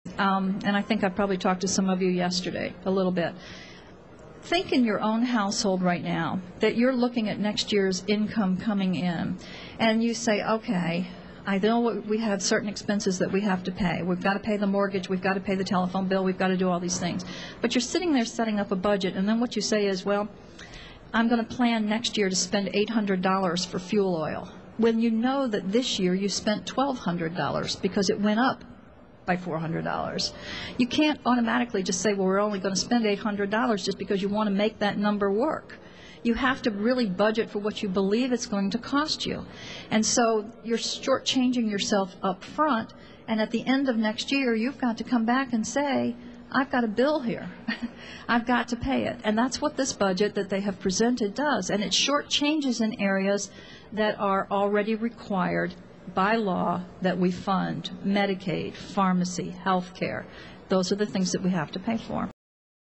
Governor Rell Addresses Proposed Democrat Budget
Connecticut Governor M. Jodi Rell (R) addresses the proposed budget by legislative Democrats in the Appropriations Committee of the General Assembly.